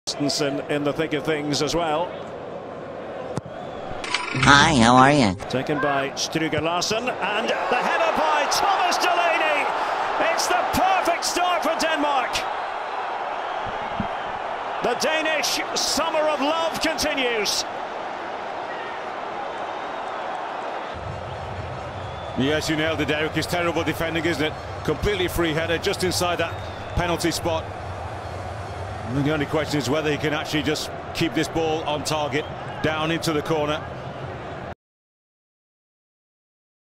Thomas Delaney opens the scoring for Denmark in the 5th
They had no clue he was there